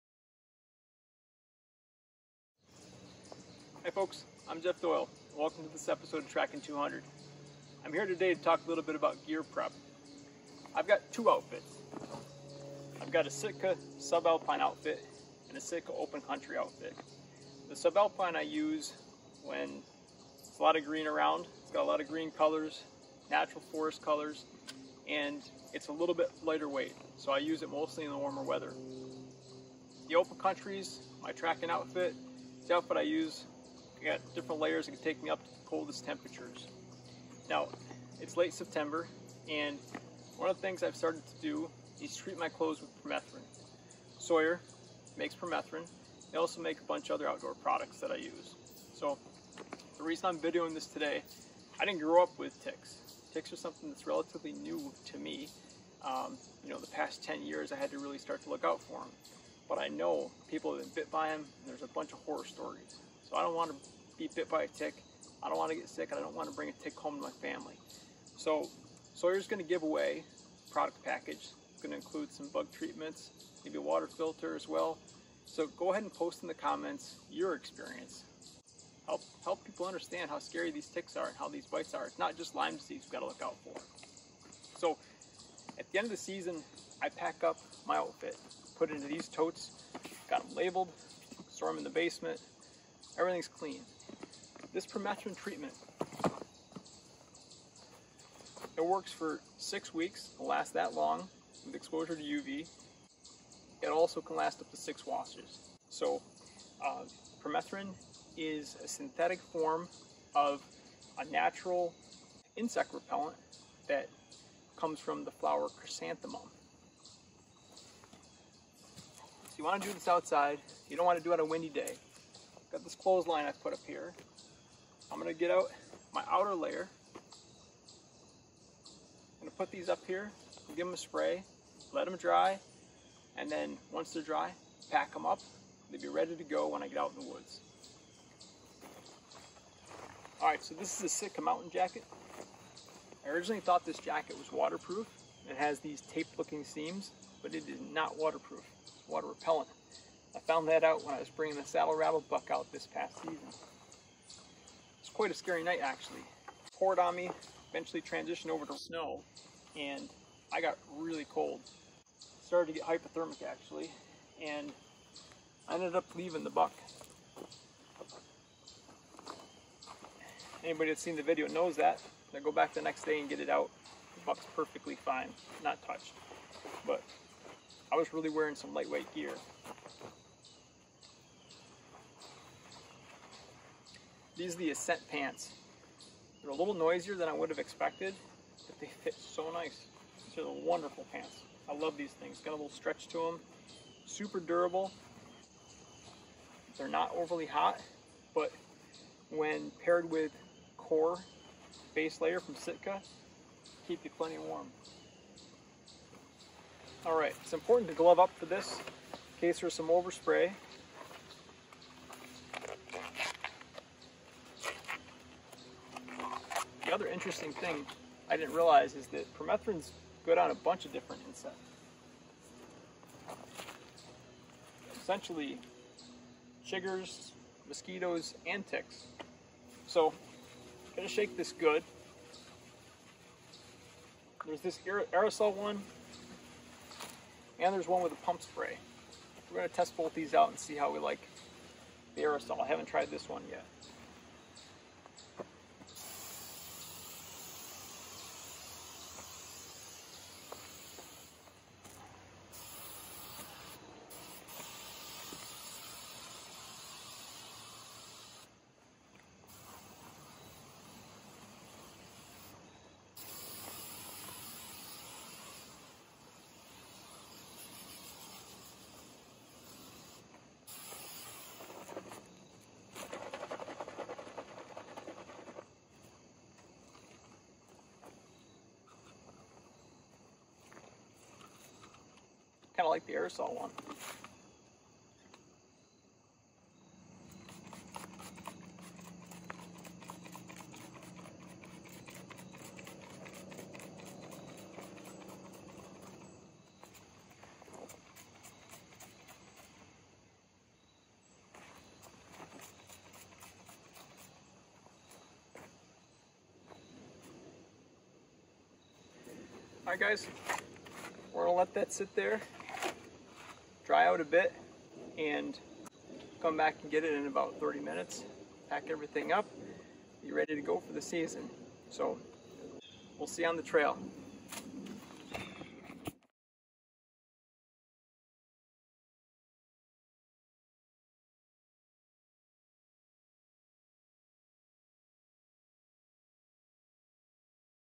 Deer Hunting Preseason Permethrin Tick Treatment of Hunting Clothes, Sawyer Product Giveaway
Channel support from:

If you like the orange and red stock ask for the "Tracking 200" edition.

Unrefined Custom Handcrafted Art
Photos on Wood